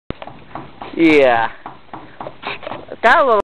Megavideos - Stuck bike
short vid becasue camera died but man ray got stuck in a bridge